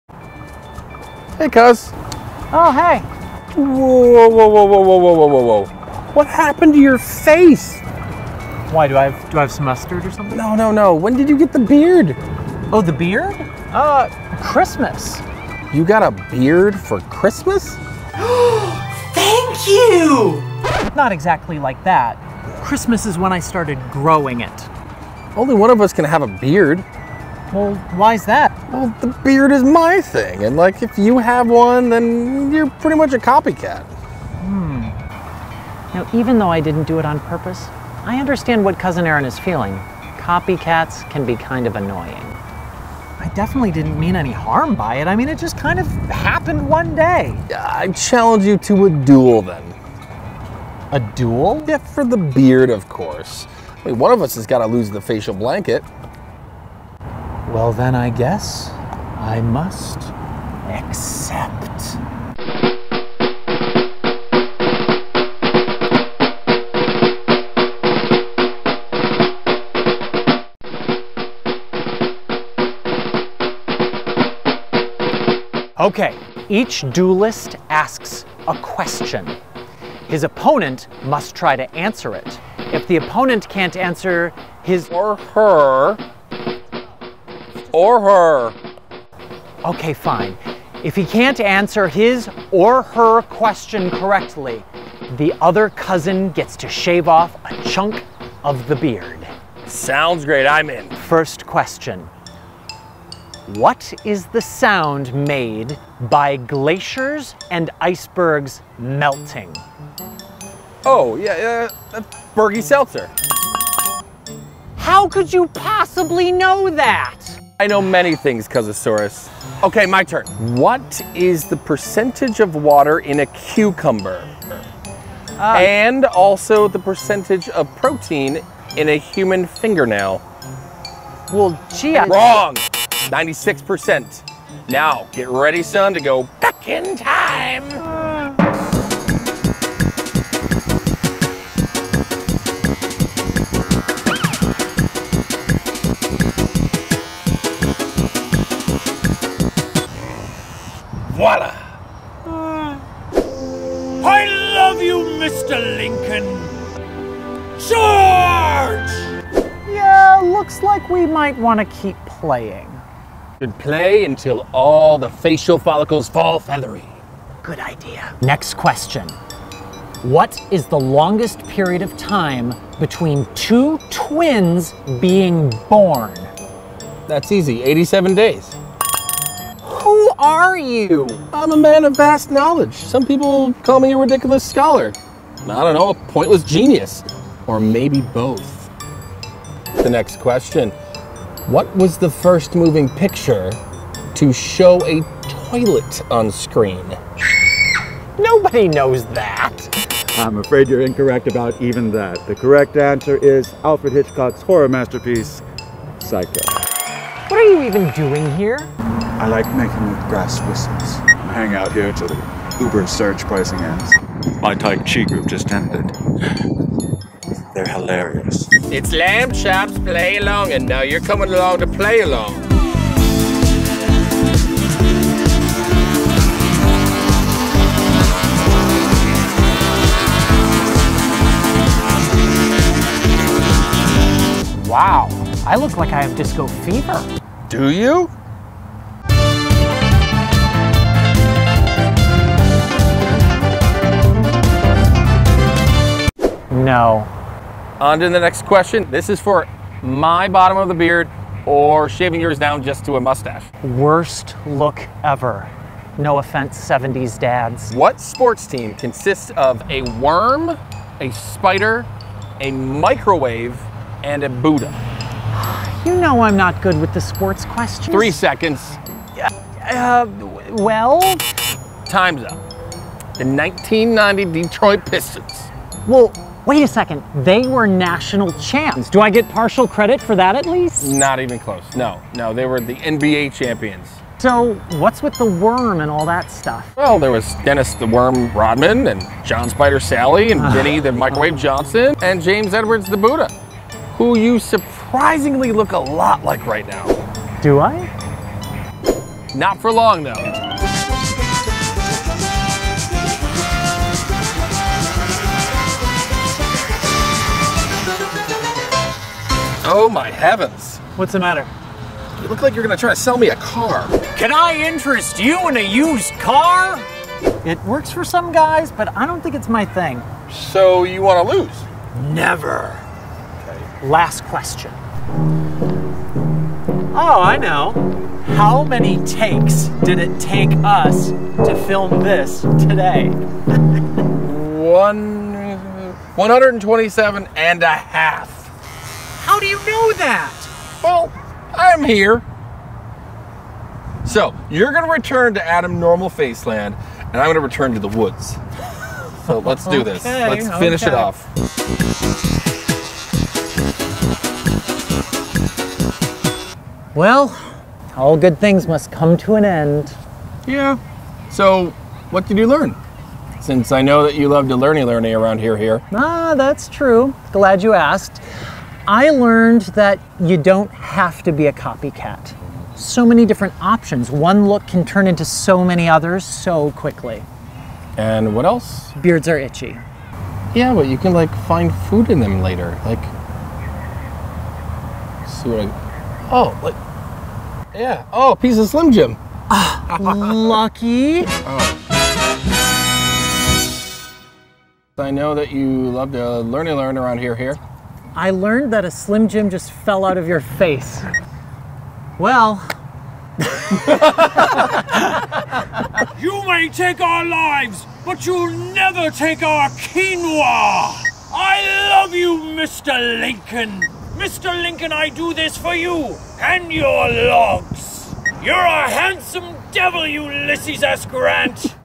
Loser Must Shave His Beard! | Adam's Answers
Cousin Aaron accuses Adam of being a "Beard Copycat" and accepts Aaron's challenge to a duel! It's the Great Beard Showdown of 2018!